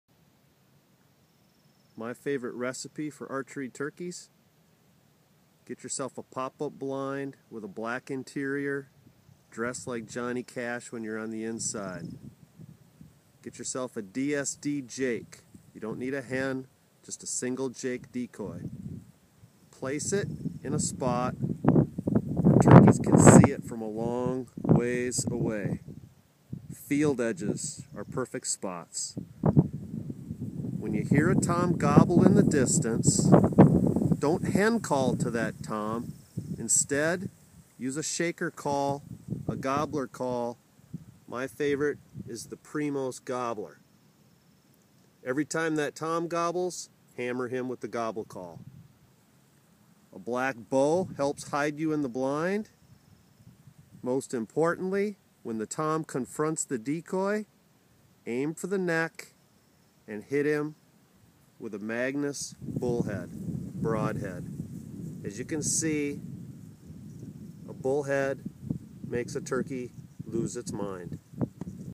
A Killer Recipe for Bowhunting Gobblers
Looking to lure toms into bow range? OutdoorHub Managing Editor Dave Maas has a proven and simple system.